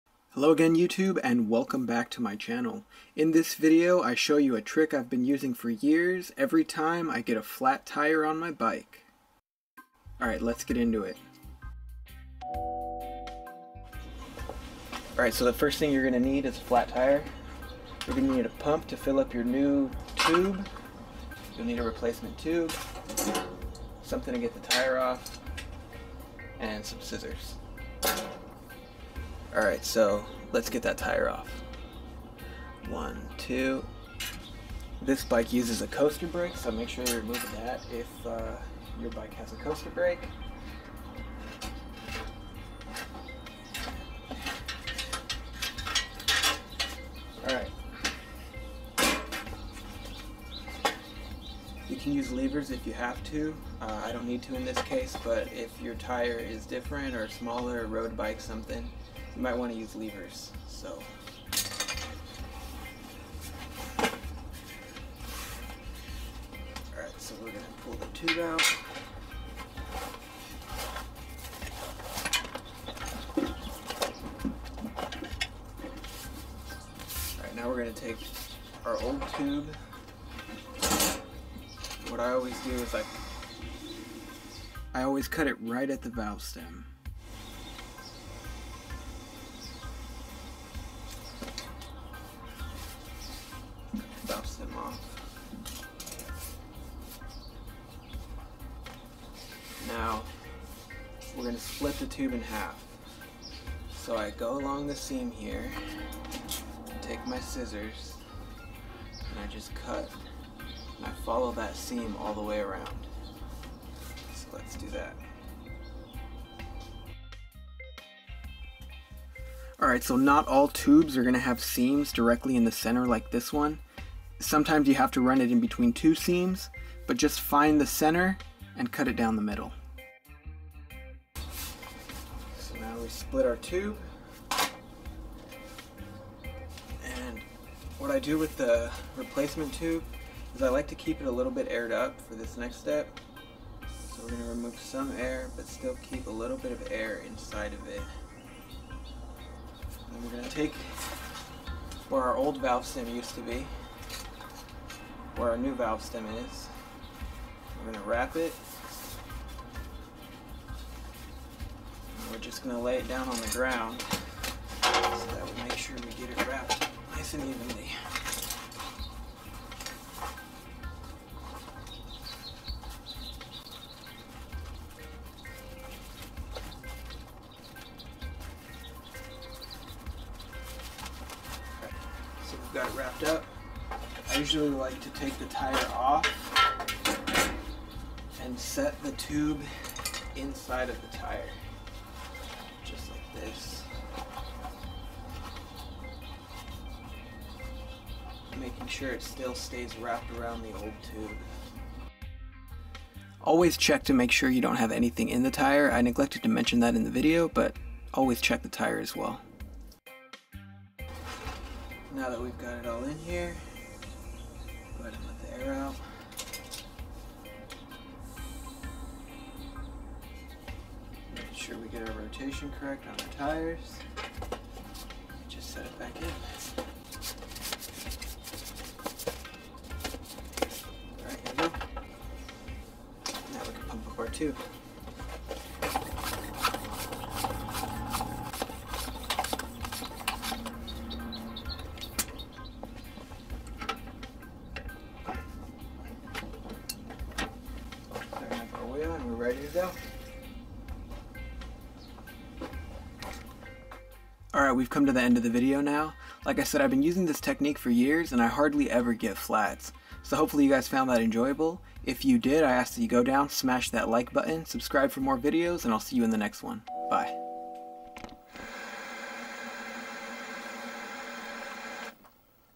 Inner Tube Repair Trick -- DIY Thick Wall Bicycle Tube
Tired of getting flats on your bike? Flat tires are an unfortunate consequence of cycling. Flats cost you time on the bike, time from your regular life, and money. Worst of all are road side flats. I've dealt with plenty of flats over the years and picked up this trick along the way to save me time and money. This double wrapped tube trick works great as an inner tube liner on all sizes of bikes: BMX, road bike, mtb, kids bikes, beach cruisers, all frames and wheel sizes from 6" to 700c. This is not a permanent fix but it will keep you on the road much longer in between flats.

***TOOLS YOULL NEED***
1 flat tube
1 replacement tube
1 wrench/ratchet to remove/install the wheel
1 pair of scissors
1 bicycle pump

***PROCEDURE***
Remove the wheel and and flat tube.
Cut the old tube at the valve stem and cut along the inner seams to split the tube in half.
Insert the replacement tube into the old tube, making sure it wraps evenly.
Check the tire for metal splinters or thorns, remove as required.
Insert the double tube, into the tire.
Reinstall the wheel and secure it with your wrench/ratchet.
You're ready to roll.

Hopefully this trick will help you save money when fixing your bike. I've used this technique for many years and I make a point to fix all family/friends bikes this way as well. Stay safe onthe road and always wear a helmet.